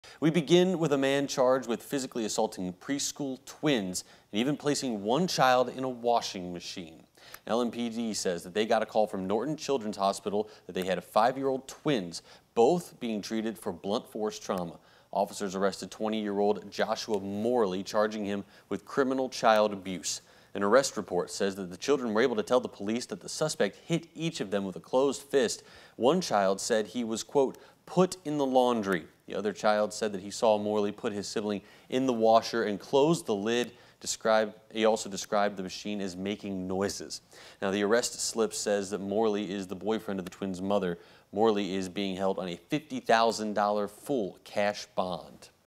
Louisville man charged with physically assaulting 5-year-old twins, placing one child in washer
Joshua Morley, 20, allegedly held each of the twins by their hair and punched them several times in the genitals and forehead.